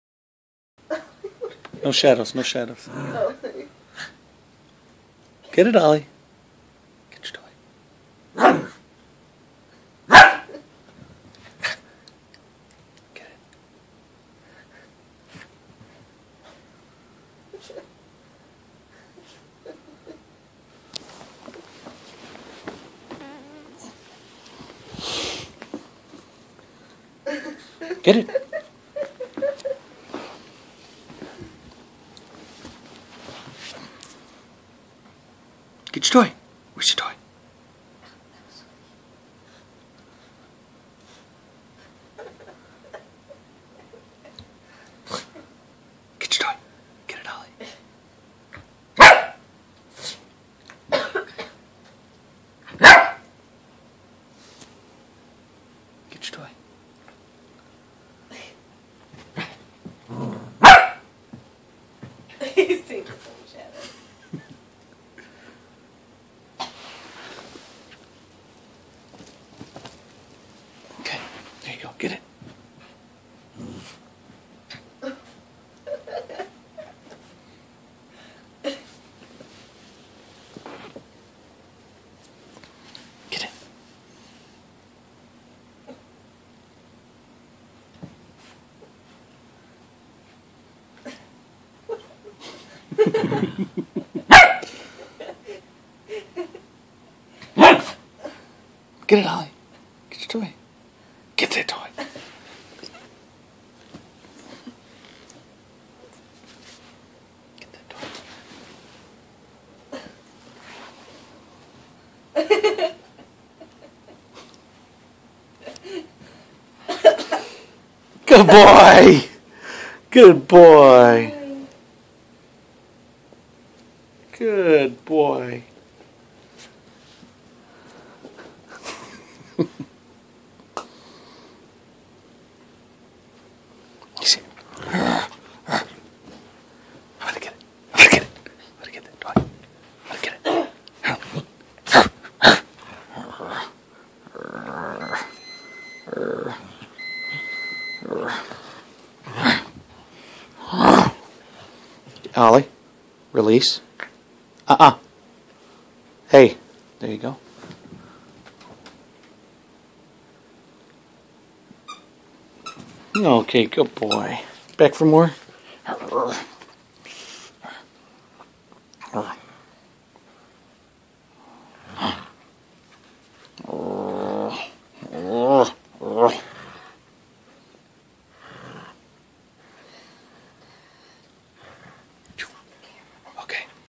Oliver, the toy and the laundry basket.
Oliver,  our cockapoo,  having a dilemma with the toy and the laundry basket.